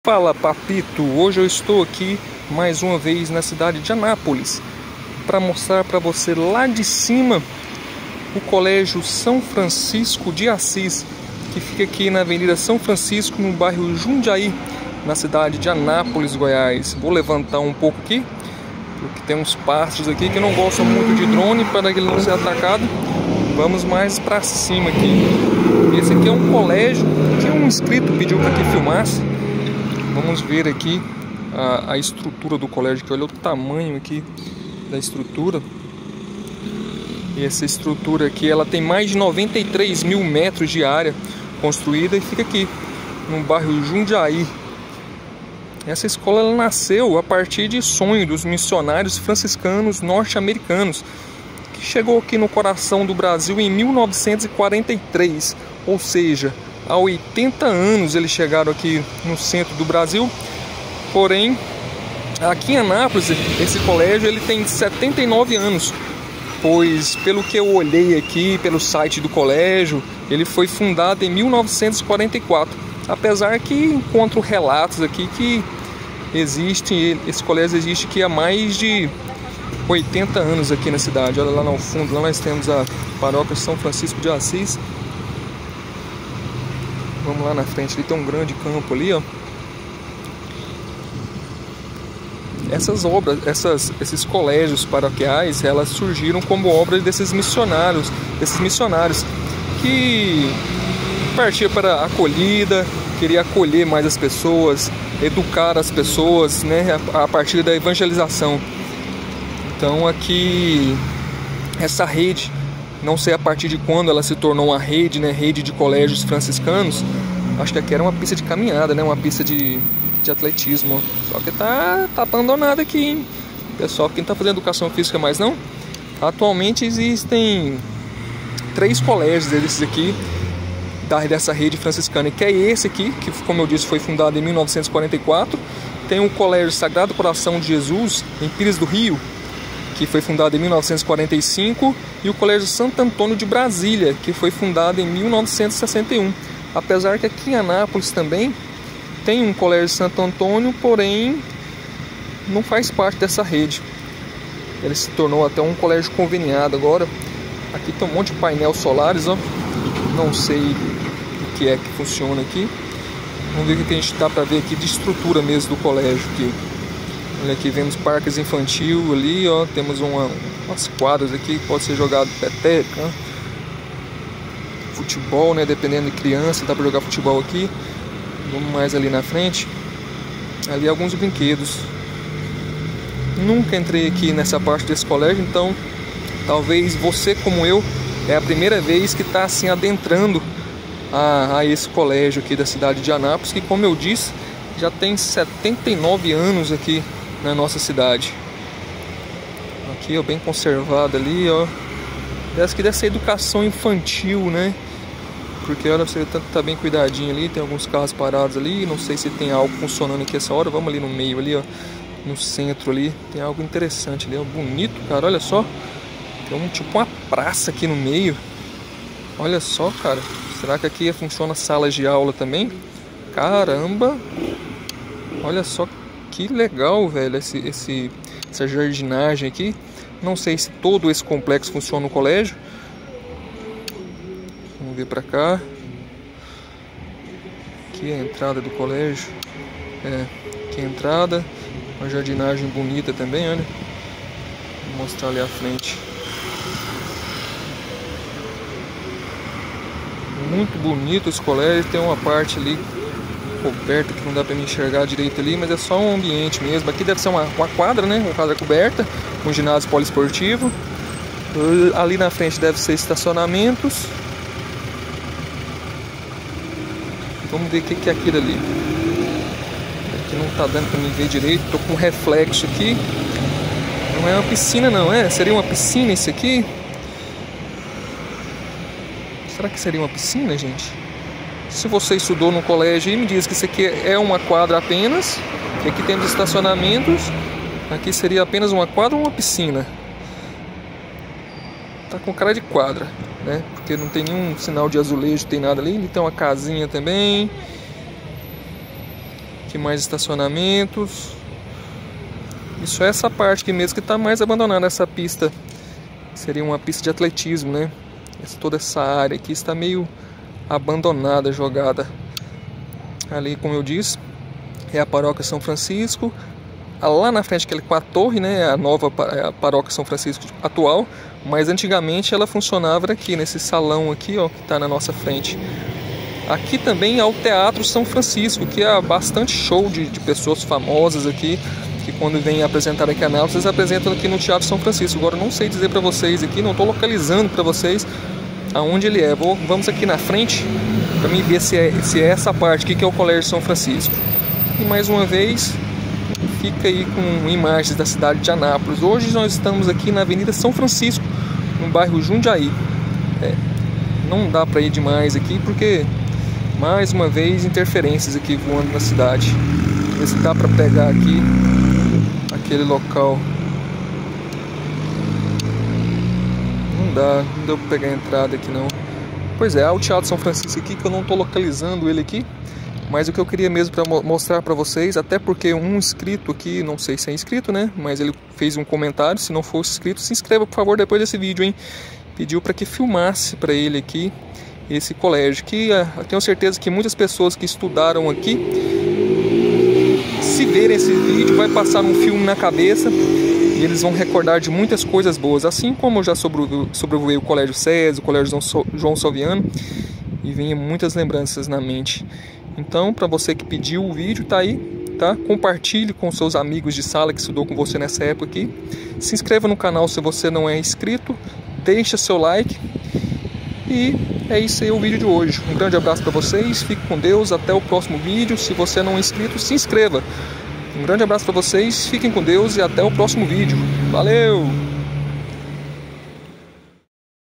Anápolis Ládecima #41 - Colégio São Franscisco de Assis
Atualmente, o Colégio São Francisco de Assis conta com uma estrutura física que está sendo modernizada ano após ano, distribuída em 93 mil metros de área, no bairro Jundiaí. Trata-se de um complexo físico muito bem projetado para atender desde as necessidades da Educação Infantil, Ensino Fundamental I e II até o Ensino Médio e tbm possui diversas áreas verdes, como a Praça de Santa Clara e o Espaço dos Flamboyants, ambientes de puro contato com a natureza. No CSFA dispomos de ótima estrutura esportiva, são duas piscinas (uma delas olímpica), duas quadras poliesportivas, um ginásio esportivo, pista de atletismo, sala de Judô, Ballet, entre outros.